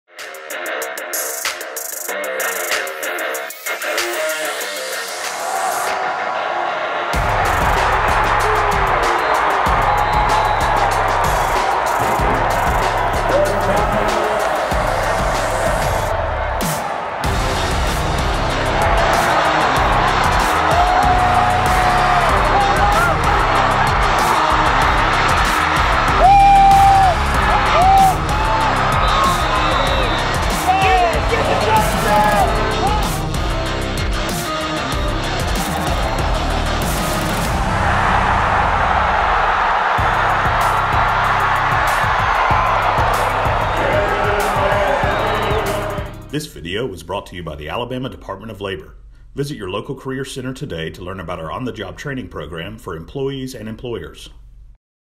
Highlights: Auburn's top 3 plays against Ole Miss
AL.com's top 3 plays from Auburn's win over Ole Miss include highlights from Boobee Whitlow, Seth Williams and Sherwood. (Wesley Sinor / )